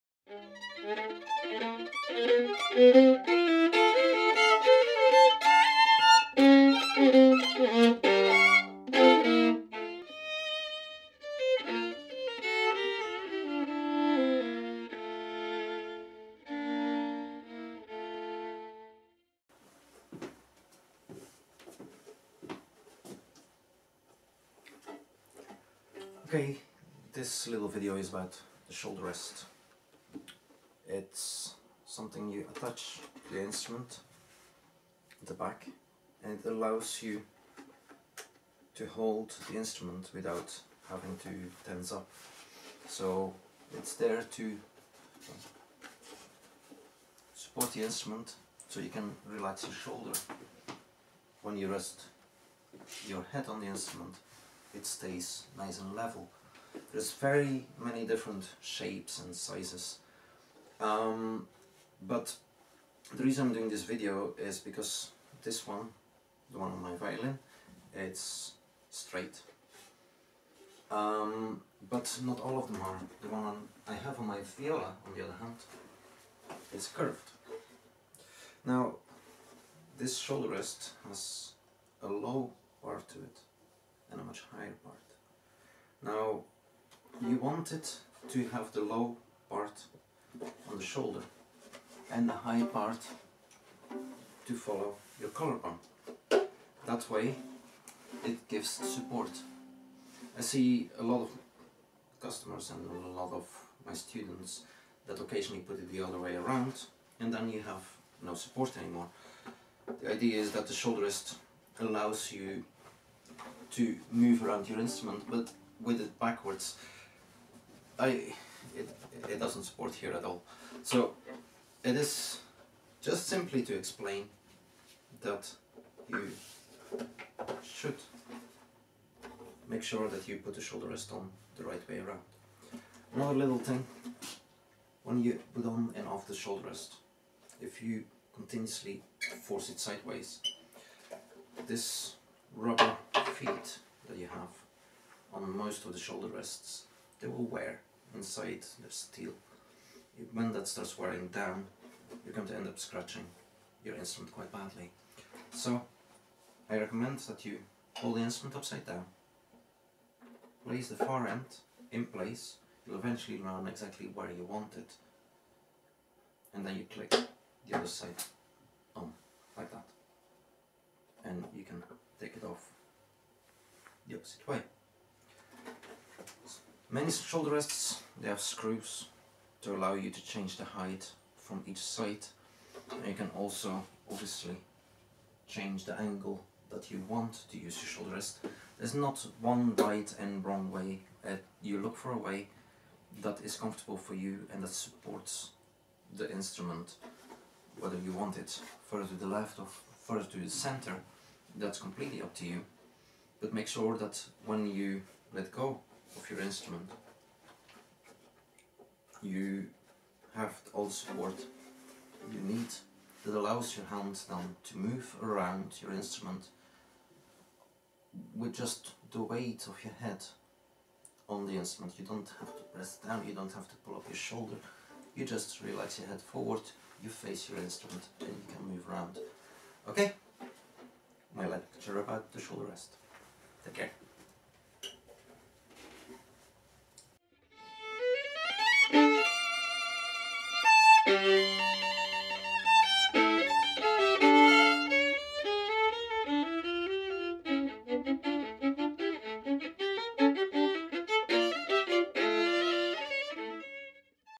The Shoulder Rest
What is a shoulder rest and why and how to use it.

Or book a lesson through Artis Futura